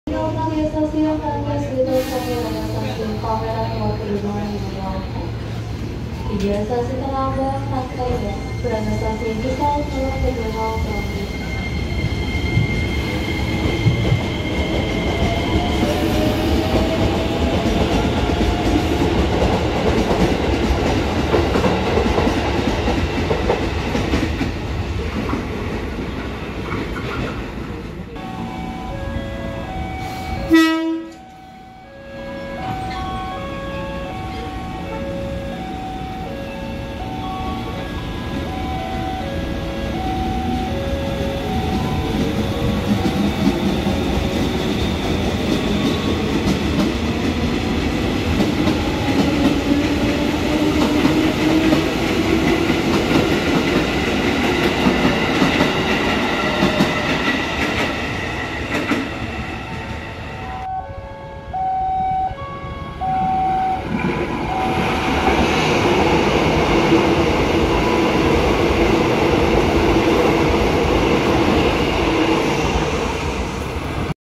Jakarta KRL and Airport Train Compilation | Part 1
A compilation of three trains.

First one filmed in Pondok Ranji has a cool sound when accelerating and an airport train passes by on the third one.